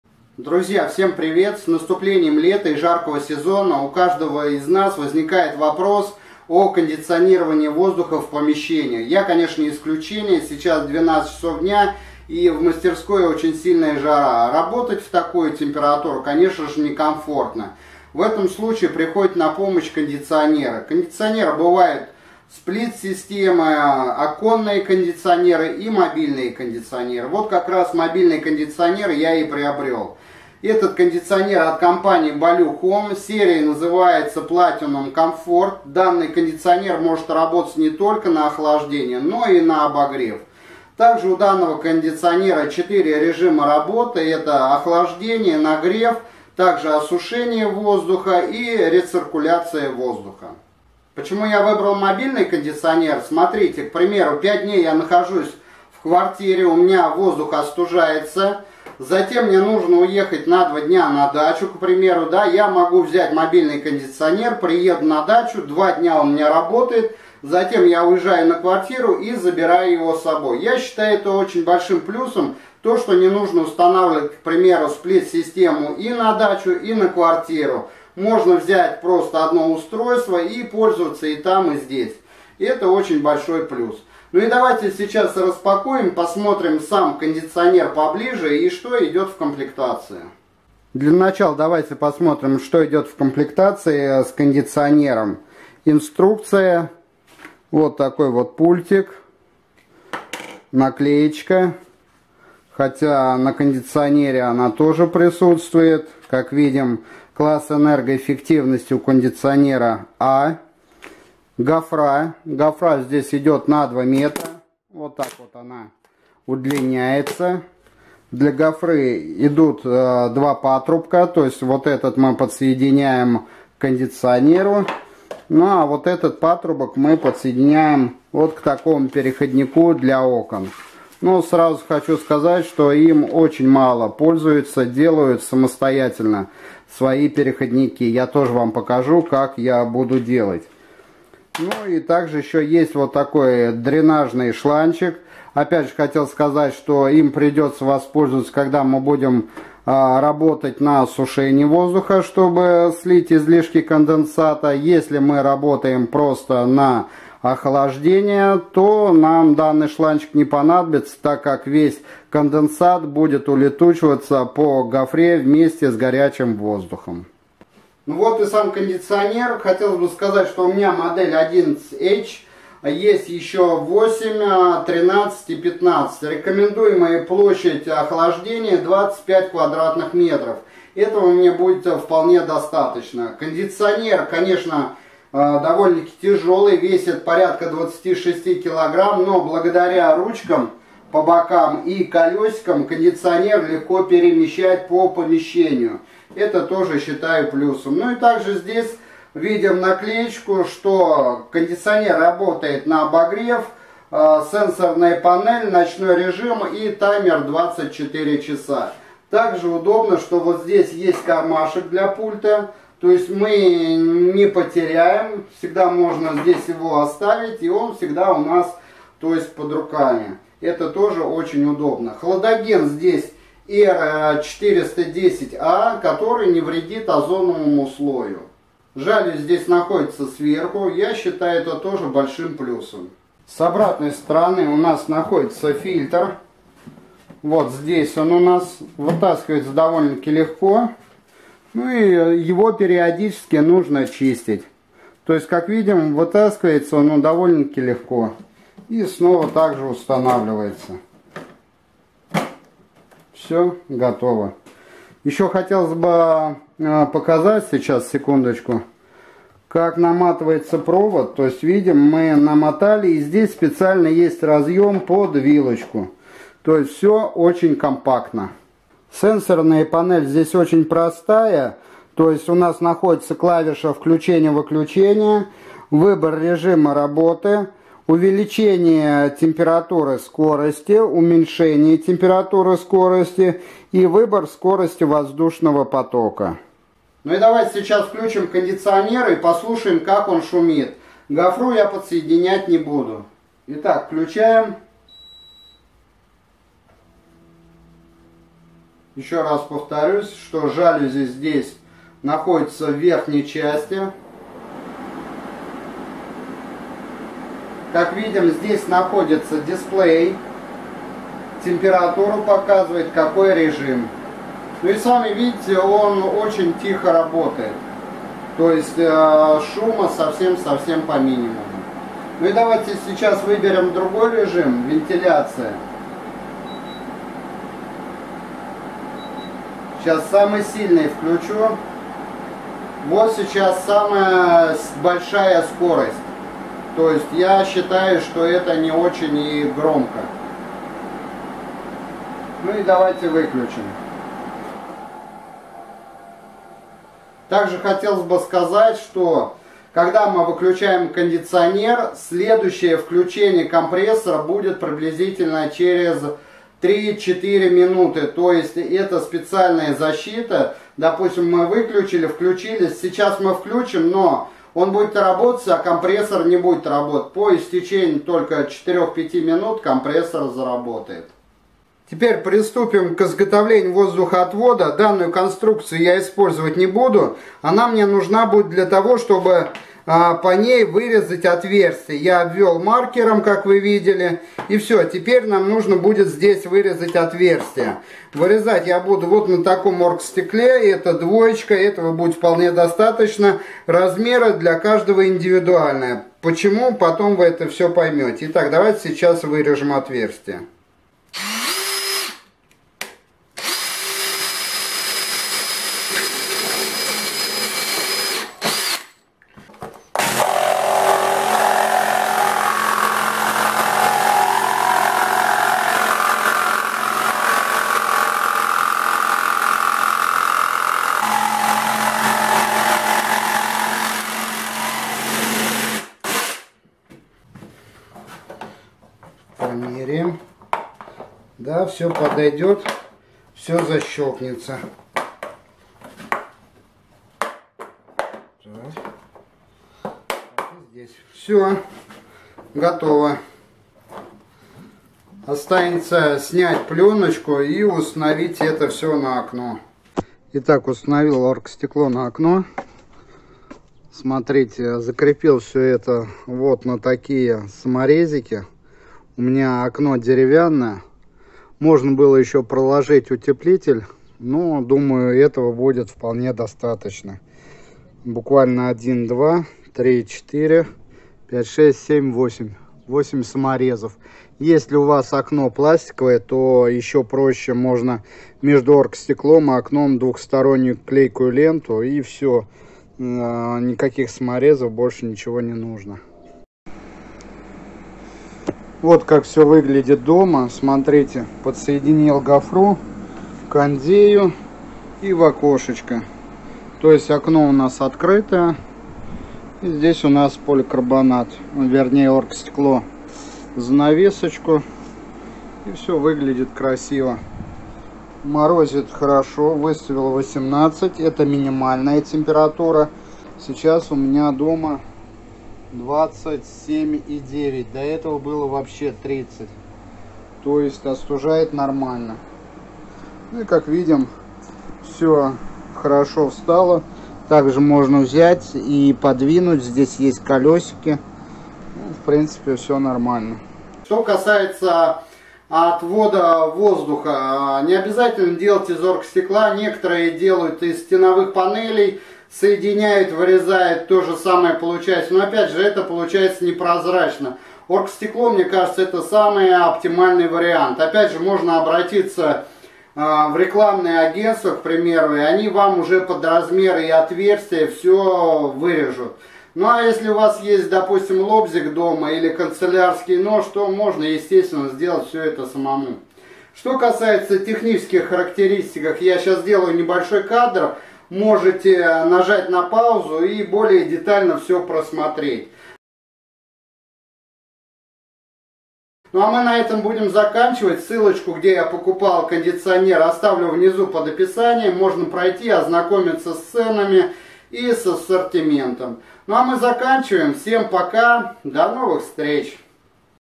Мобильный кондиционер Ballu Platinum BPHS-11H .Установка мобильного кондиционера своими руками
Мобильные кондиционеры Ballu серии Platinum пригодятся не только летом, но и в межсезонье, так как помимо охлаждения, осушения и проветривания воздуха могут еще и обогревать помещение.
Полезные советы, идеи, хитрости старых мастеров, Станки и приспособления, самоделки,  ждут тебя!